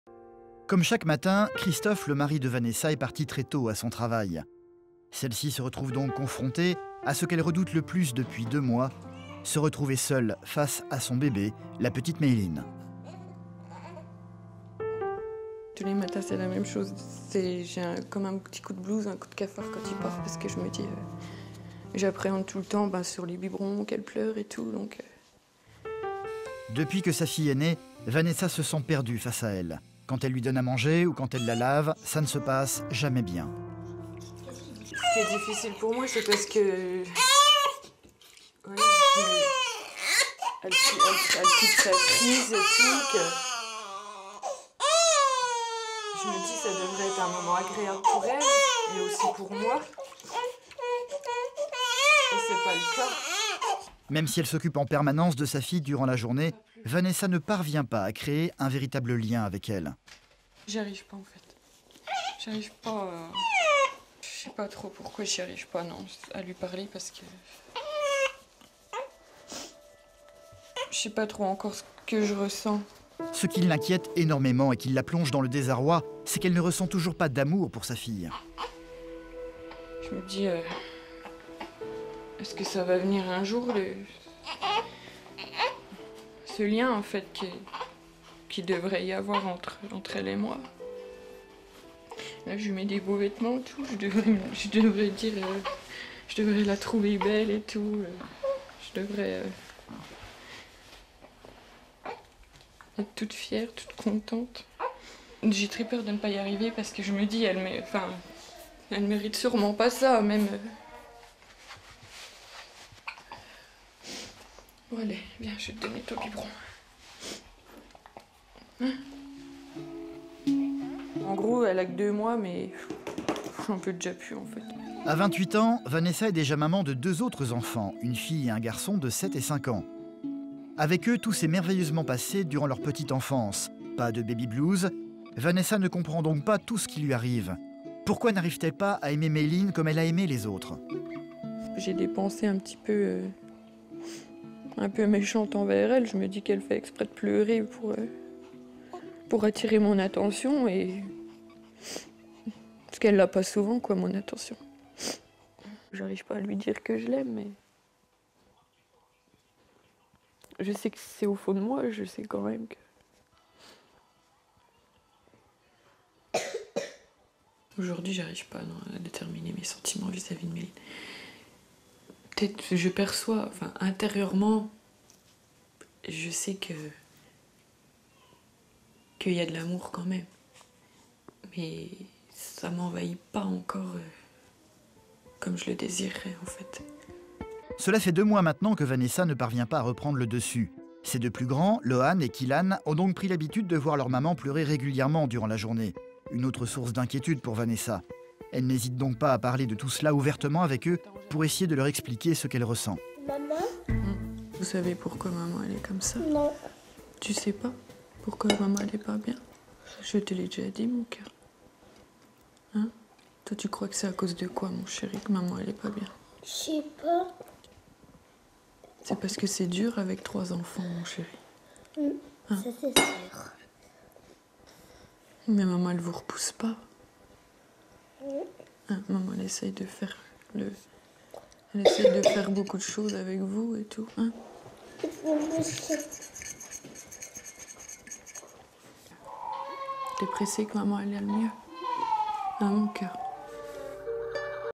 Elle a un baby blues avec son troisième enfant
Se retrouver seule face à son troisième enfant est une véritable source d'angoisse